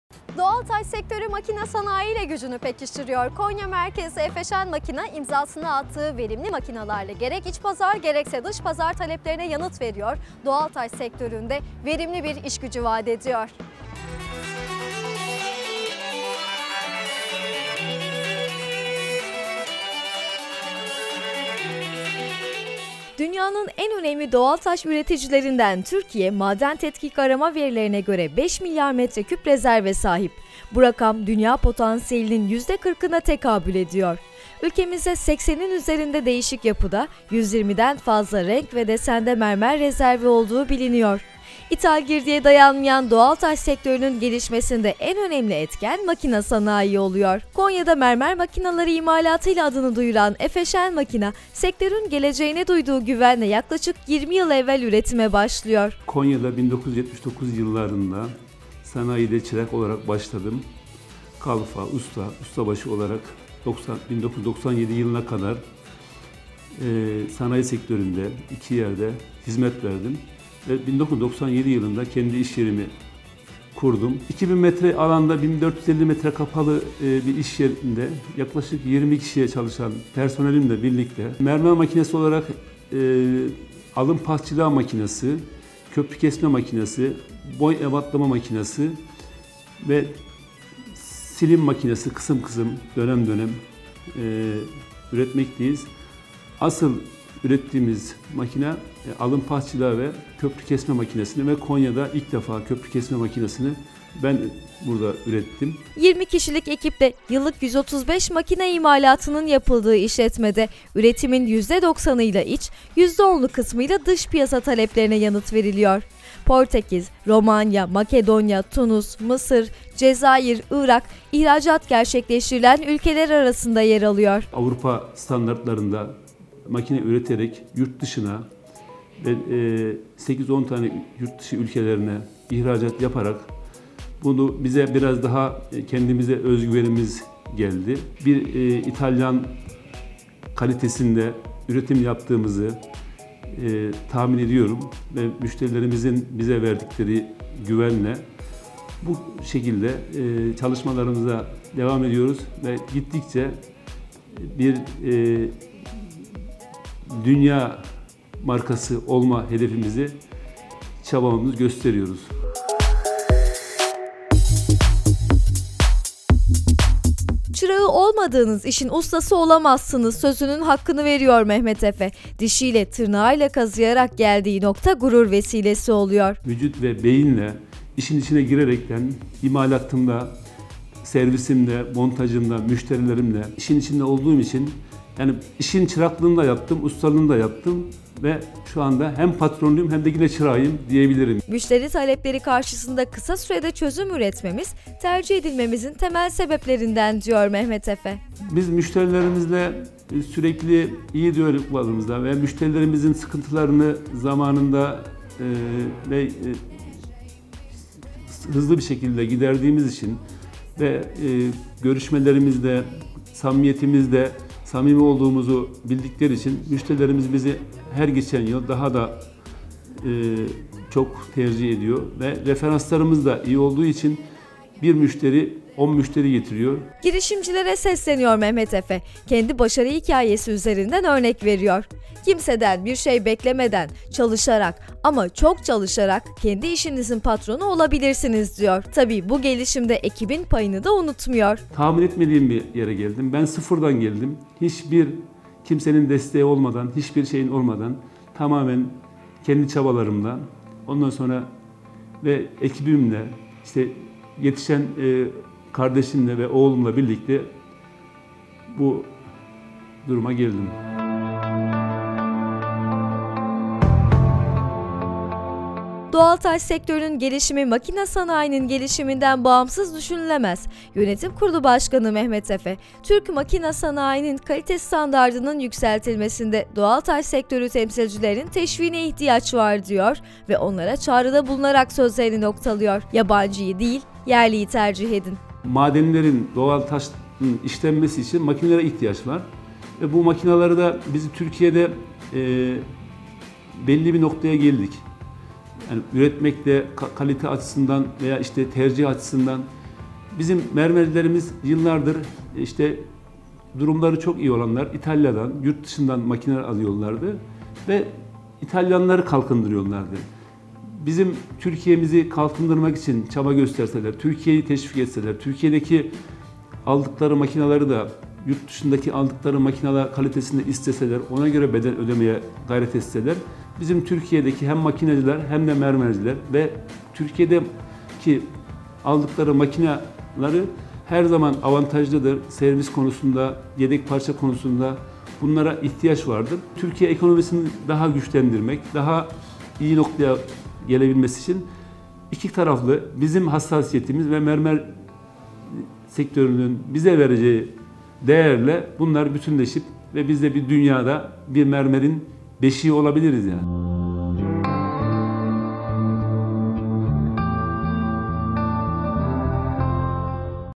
Efeşen Makina
Doğaltaş sektörü, makine sanayiyle gücünü pekiştiriyor. Konya merkezli Efeşen Makine , imzasını attığı nitelikli makinalarla gerek iç gerek dış pazar taleplerine yanıt veriyor, doğaltaş sektörüne  verimli iş gücü vaad ediyor..